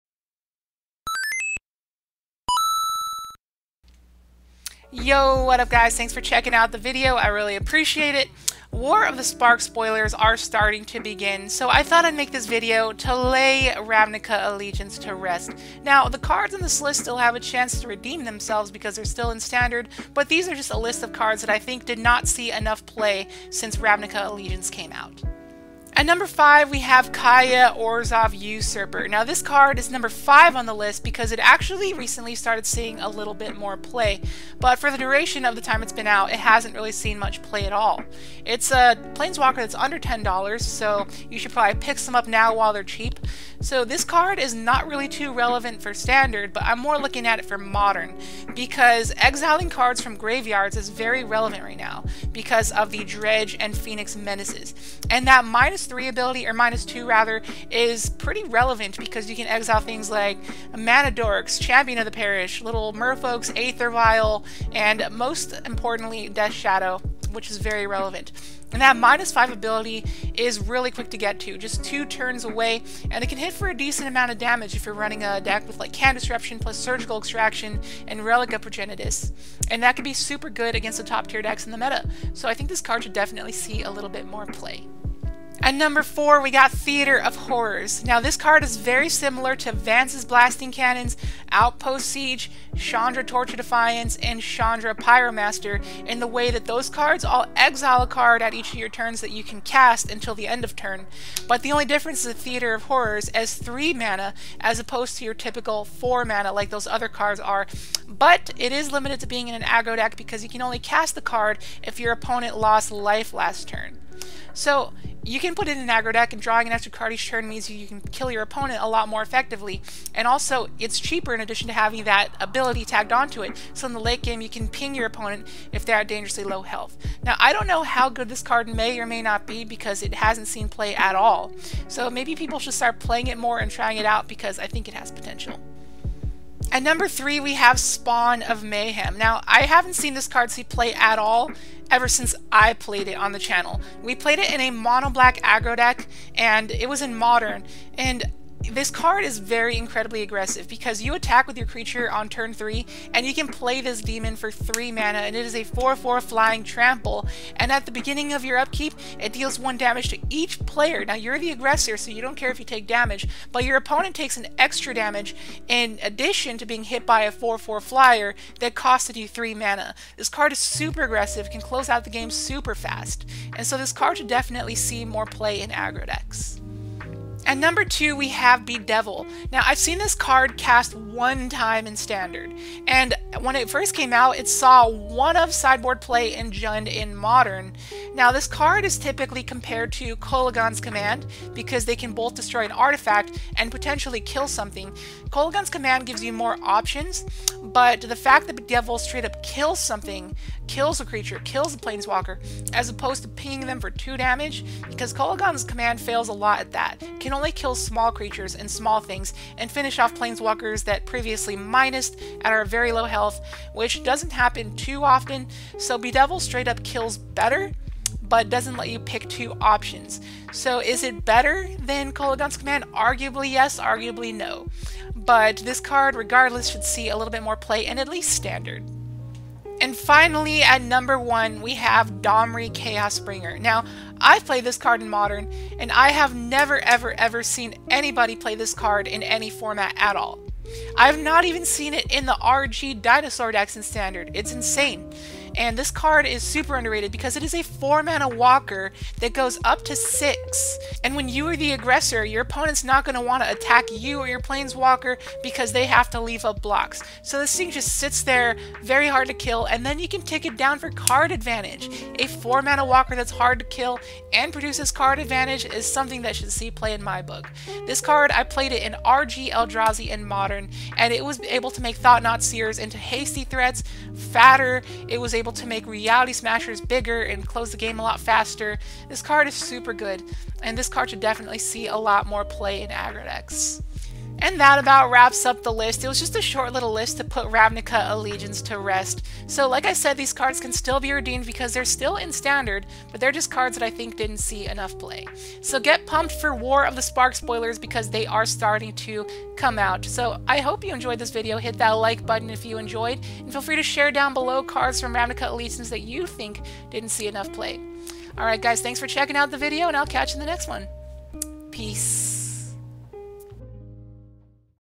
Top 5 Ravnica Allegiance cards that didn't see enough play
Ravnica Allegiance has lived its hype phase out, but some cards were left in the dark corner. Let's bring them into the spotlight and see which cards didn't see enough play.